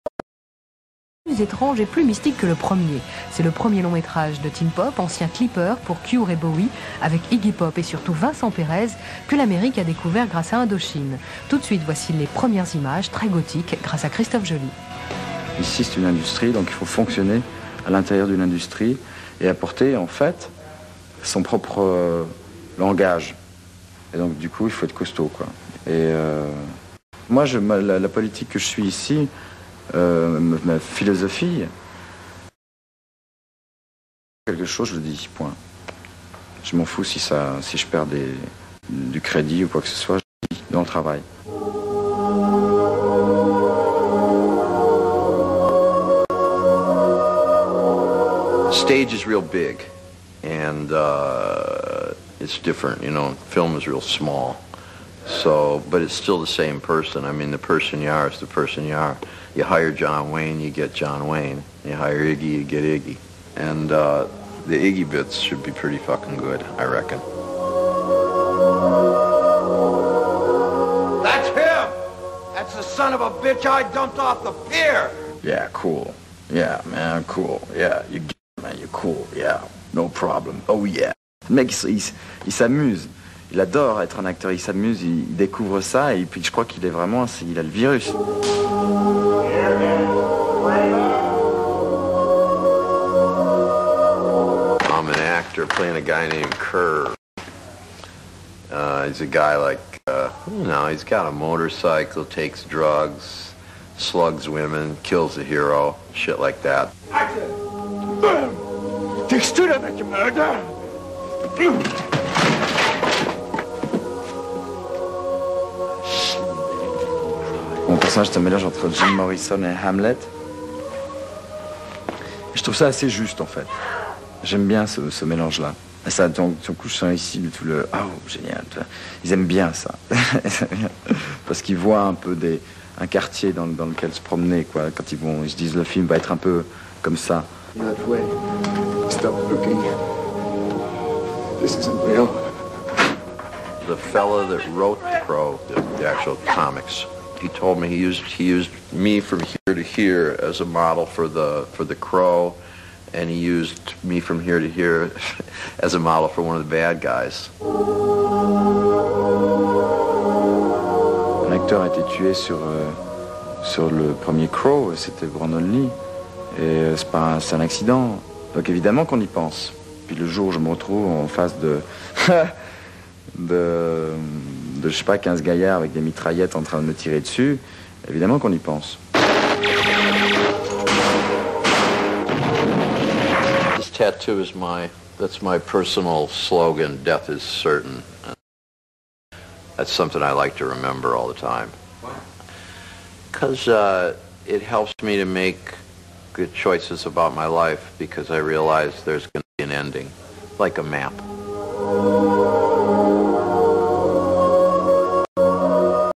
Iggy Pop & Vincent Perez talk about the making of The Crow City of Angels 1996 French TV English sub
Iggy Pop & Vincent Perez talk about the making of The Crow City of Angels on a French TV special back in 1996. English subtitles are on there for the majority of it